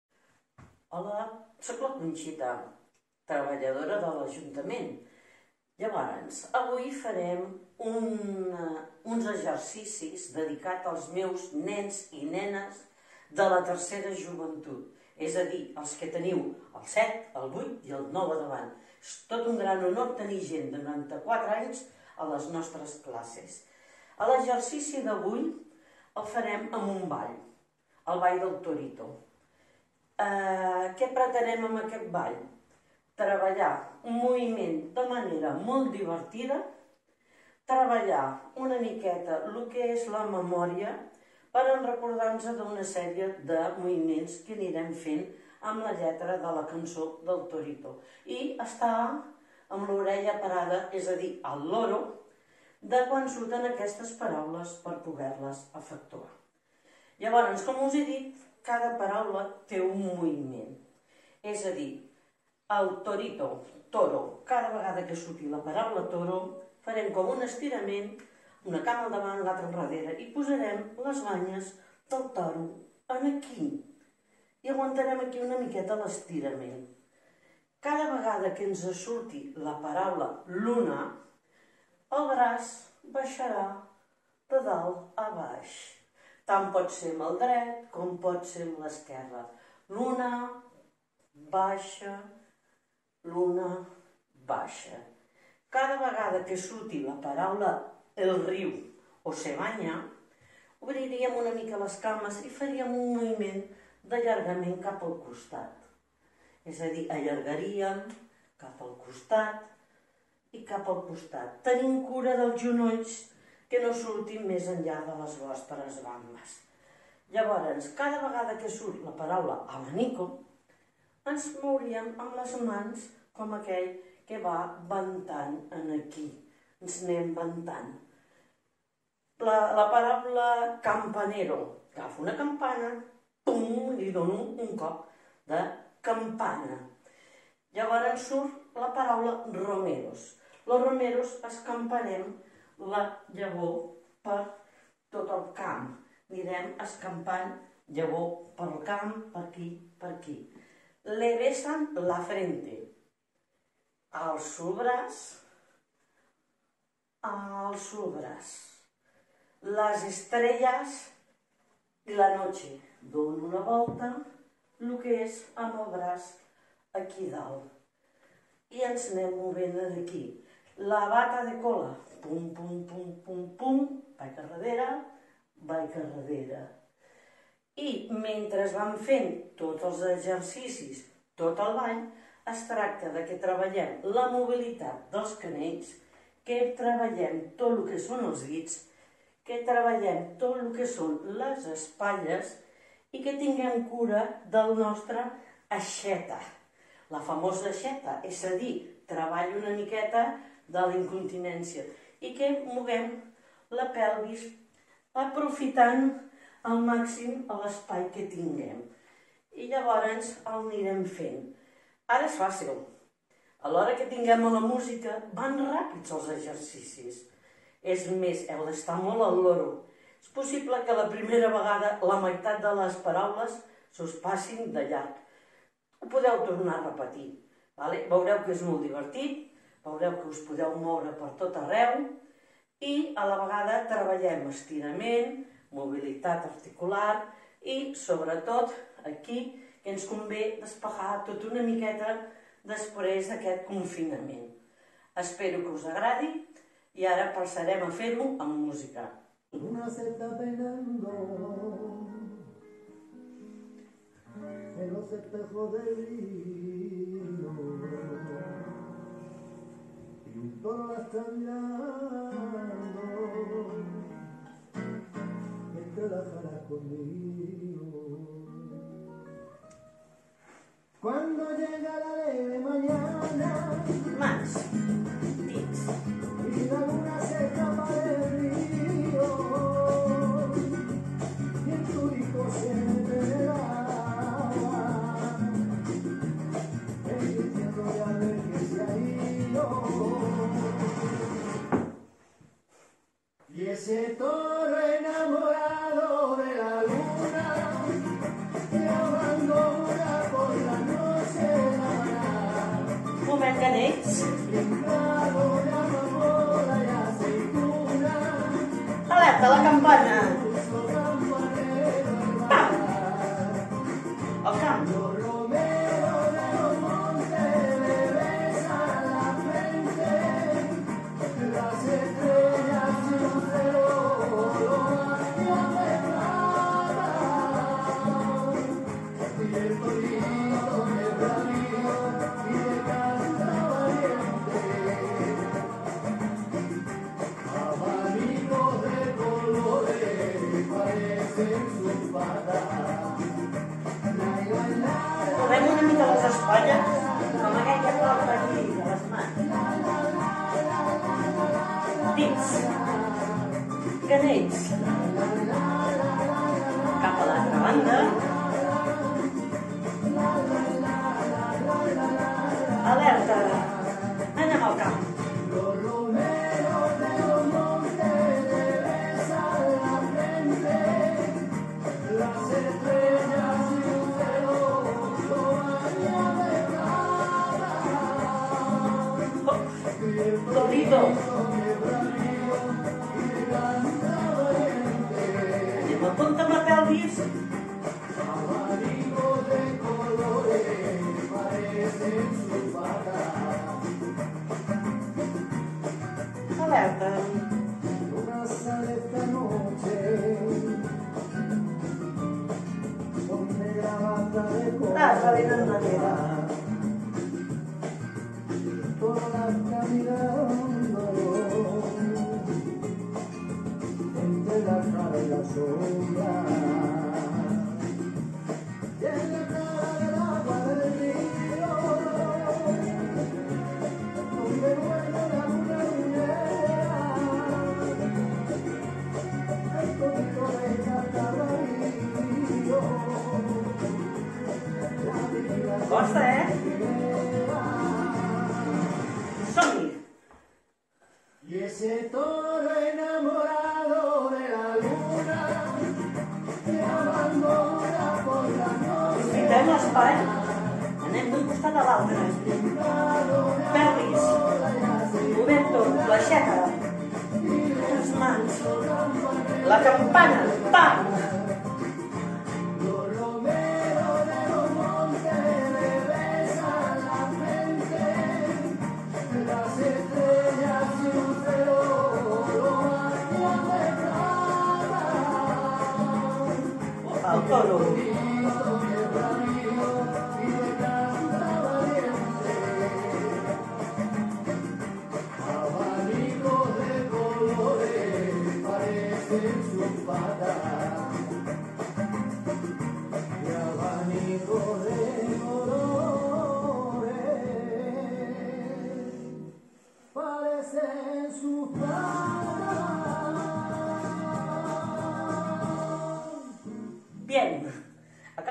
Píndoles d'activitat física 01 Dolça_Coreo 1
Des de la Secció d’Esports de l’Ajuntament de Manresa, volem contribuir a que els alumnes del programa d’activitats fisicoesportives per adults MES (Manresa, Esport i Salut) pugueu tenir un confinament actiu. És per això que diàriament anirem oferint «píndoles d’activitat física» adreçades a vosaltres.